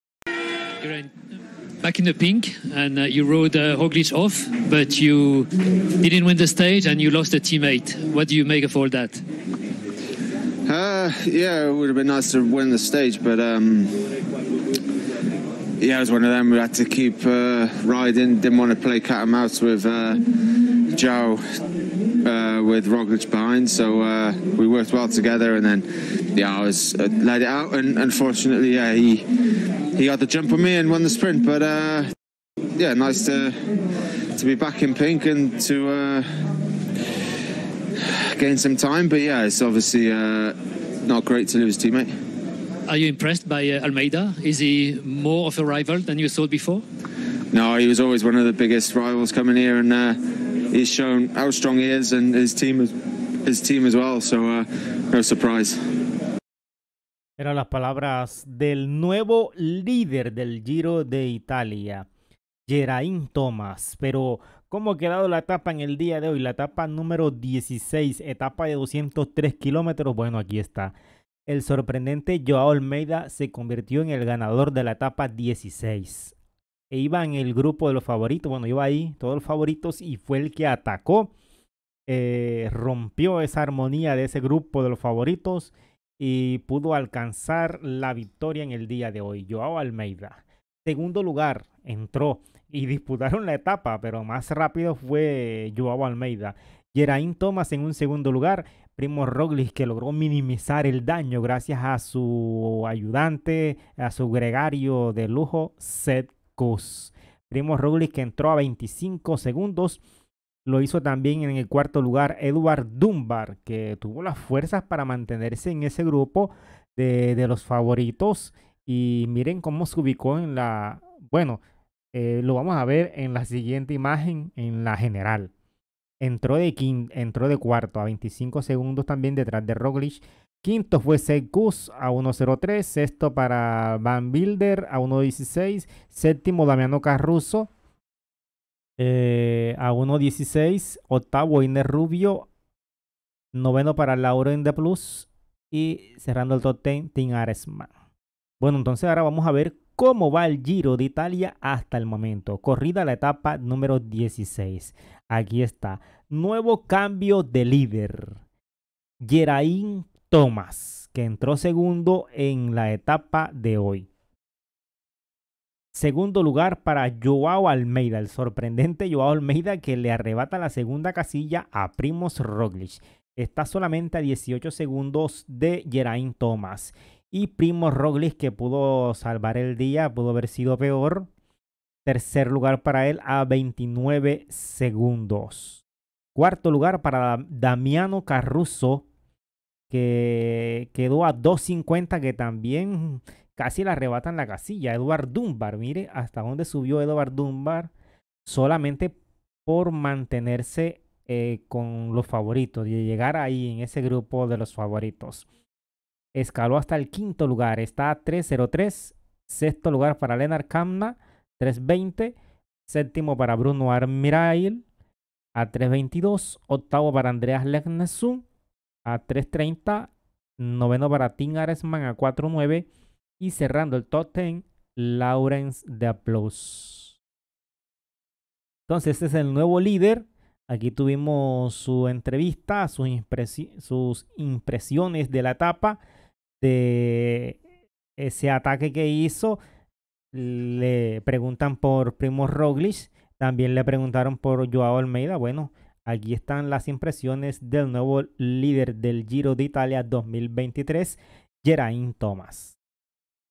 GERAINT THOMAS - REACTION AT THE FINISH - REACCION TRAS SER NUEVO LIDER DEL GIRO DE ITALIA ETAPA 16
LA REACCION DEL CORREDOR DEL INEOS GERAINT THOMAS TRAS SER NUEVO LIDER DEL GIRO DE ITALIA 2023